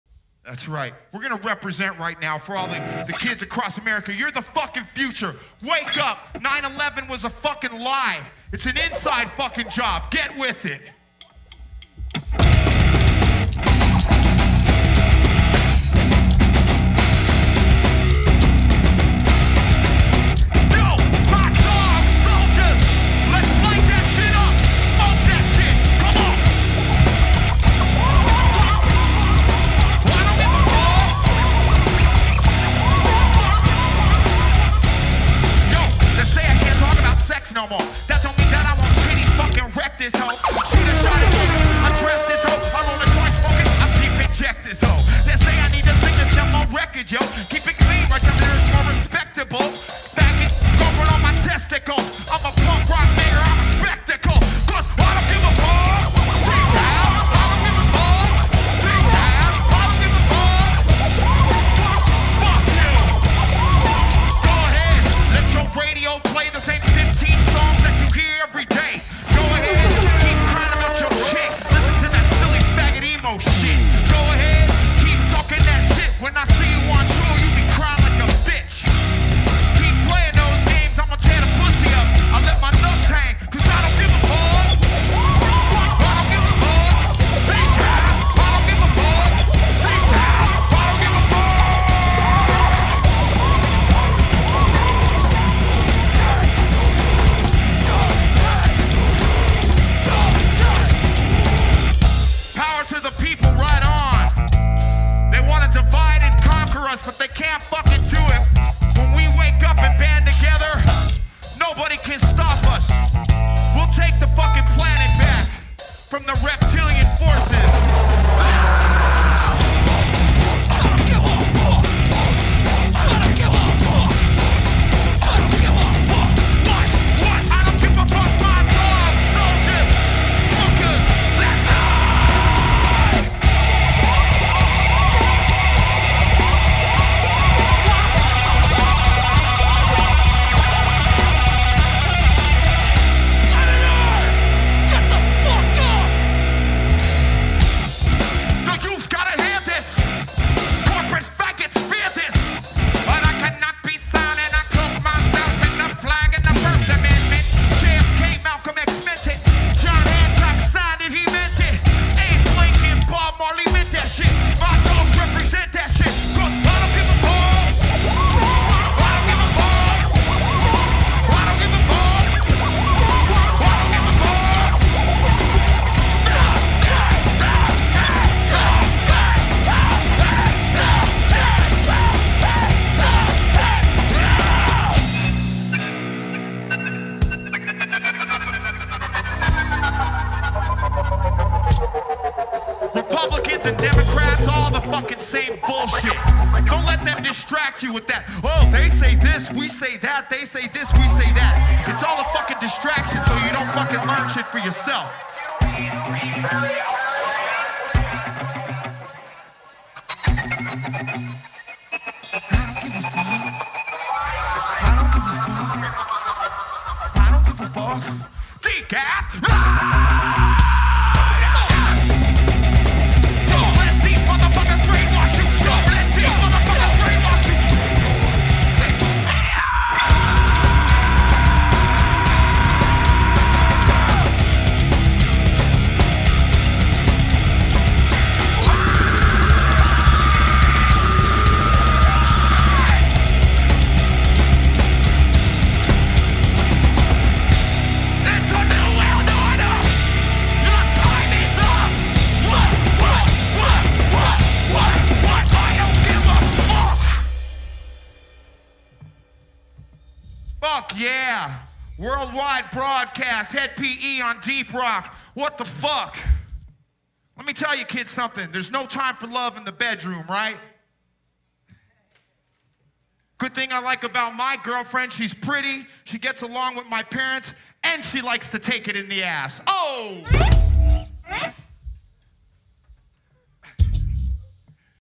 (hed) pe part7
DeepRockDrive.com - (hed)pe performs song 7/12 live at deeprockdrive.com